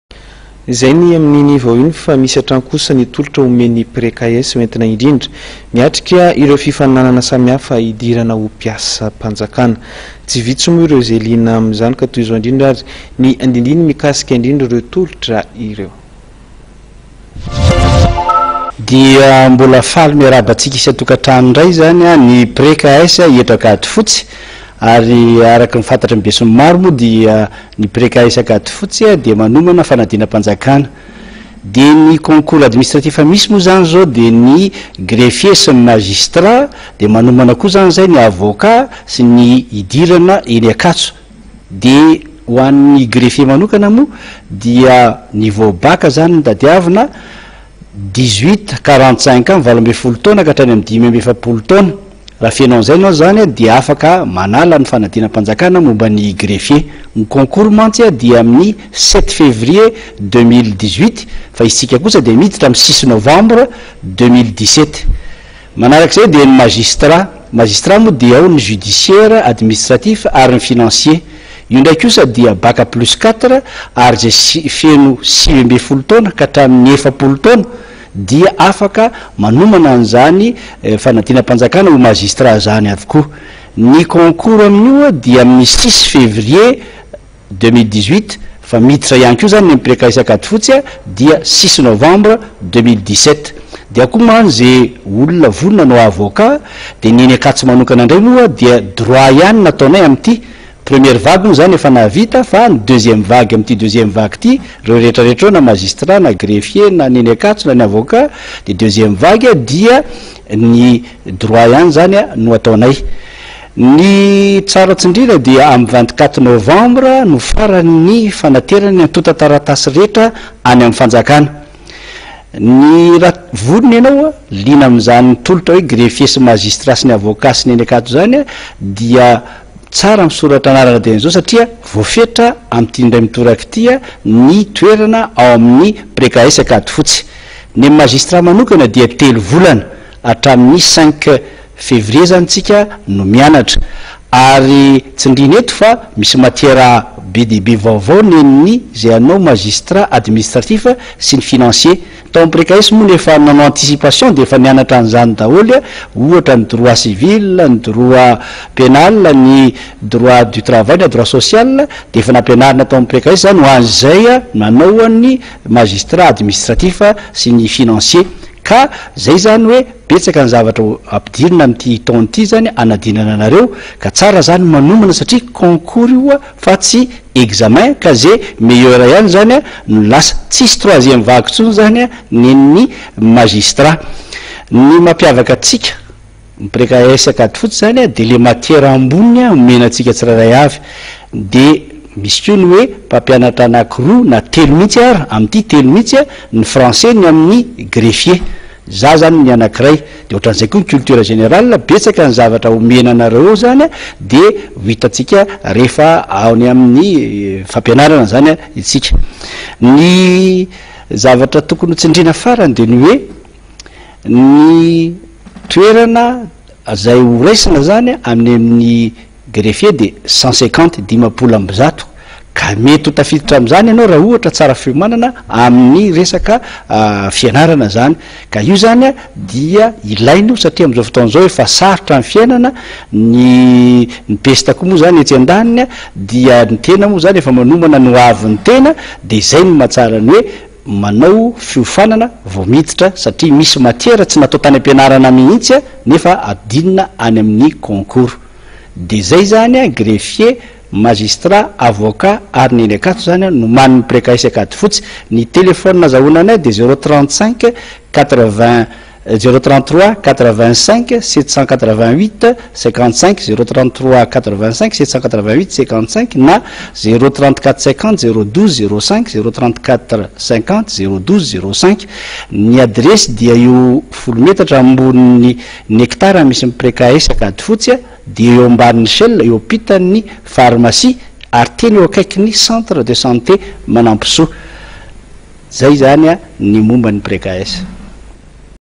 09 PRECAES
Misy hatrany ny tolotra omen'ny PRECAES hoentina miatrika ireo fifaninanana samihafa hidirana ho mpiasam-panjakana. Tsy vitsy ireo  izay liana amin'izany. Toy izao ny andinindininy mikasika ireo tolotra.